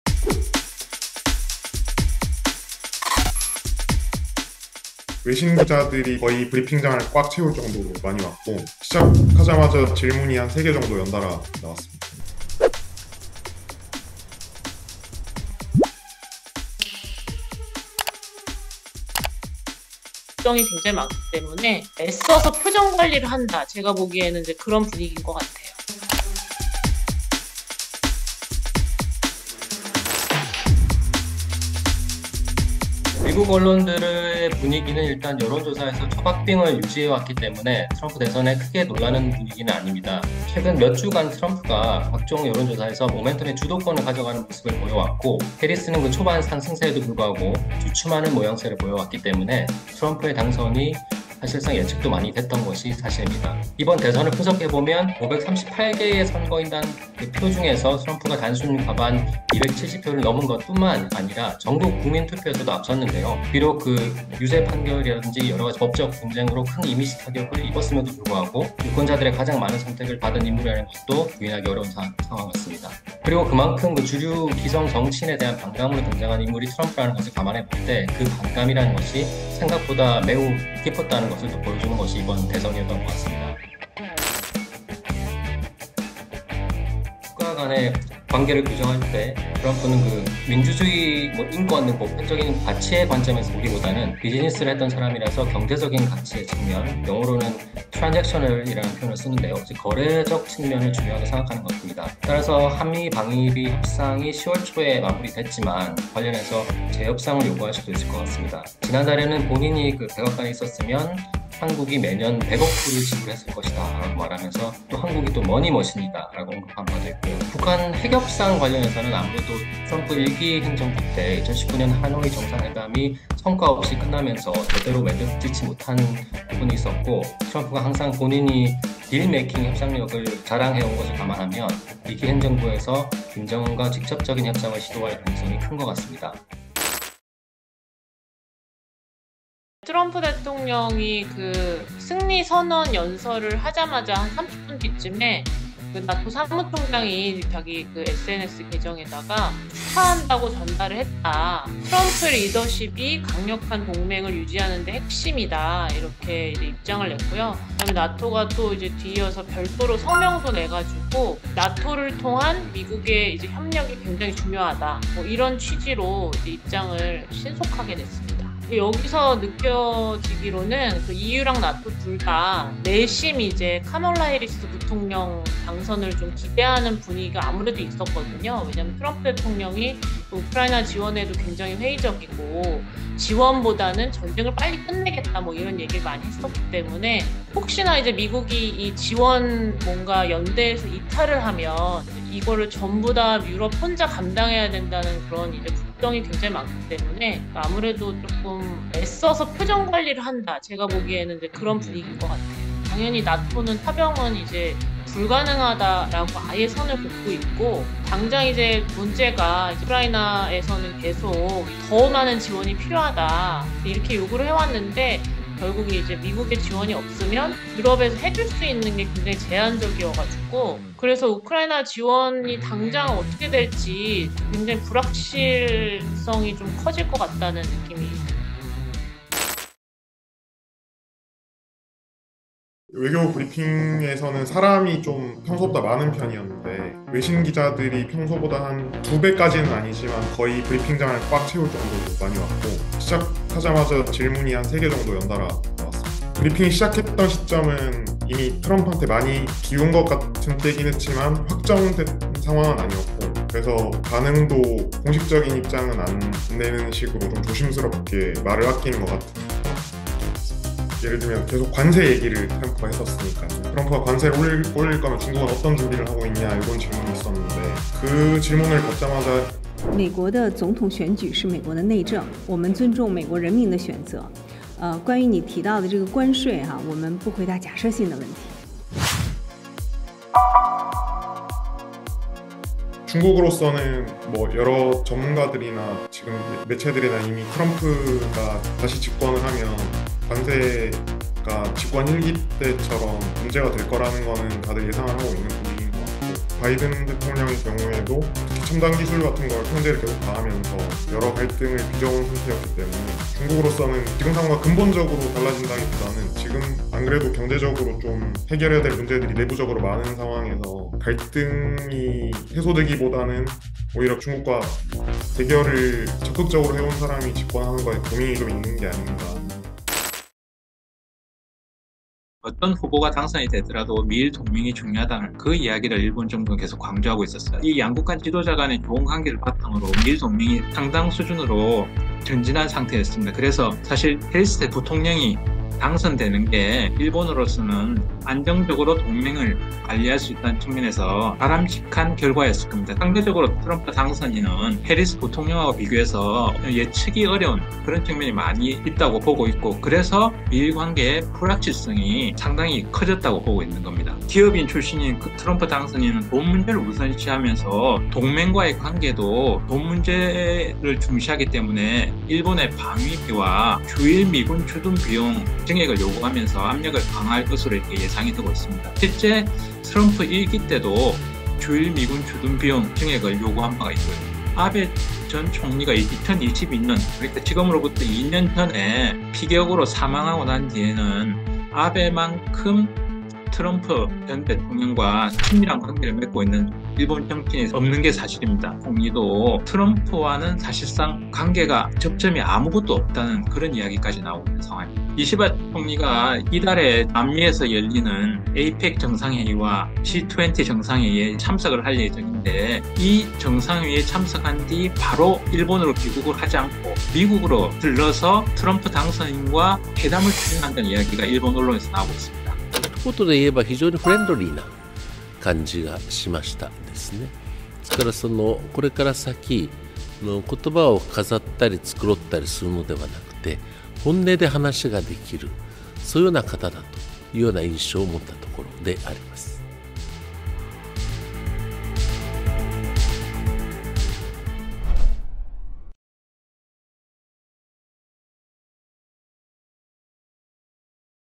[특파원 현장] 트럼프 귀환에 긴장한 세계..특파원이 전한 각국 반응 / 연합뉴스 (Yonhapnews)
(서울=연합뉴스) 도널드 트럼프 전 대통령의 백악관 귀환이 확정되면서 트럼프 2기를 맞는 각국의 발걸음이 더욱 분주해졌습니다.
    트럼프 1기 당시 미국 정부와 마찰을 빚었던 중국과 북대서양조약기구(NATO·나토) 등은 조심스러운 입장을 유지하면서 트럼프 당선인의 향후 행보에 촉각을 곤두세우는 분위기입니다.
    당장 마르크 뤼터 나토 사무총장은 6일(현지시간) 트럼프 전 대통령이 대선 승리를 선언한 지 약 40분 만에 SNS(사회관계망서비스)를 통해 발 빠르게 당선 축하 인사를 건넸습니다.
    시진핑 중국 국가주석은 7일 트럼프 당선인에 축전을 보내 "중미가 협력하면 모두에 이롭고(合則兩利) 싸우면 모두가 다친다(鬪則俱傷)"는 뼈 있는 말로 양국 협력의 중요성을 강조했습니다.
    이시바 시게루 일본 총리는 같은 날 오전 트럼프 당선인과 통화해 이른 시일 내 만나기로 했다고 밝혔습니다.
    연합뉴스는 현지 특파원들을 통해 더욱 자세한 얘기를 들을 수 있었는데요, 영상으로 보시죠.

    0:30 미국의 선택은 예견된 결과?
    1:24 한반도와 새로운 거래?
    2:17 안보 부담 커진 유럽은 '전전긍긍'
    3:50 중국 기자실이 북적댄 이유
    5:20 발빠르게 움직이는 '아베 없는' 일본

    기획·구성: 고현실
    편집: 백지현
    영상: 로이터·트럼프 유튜브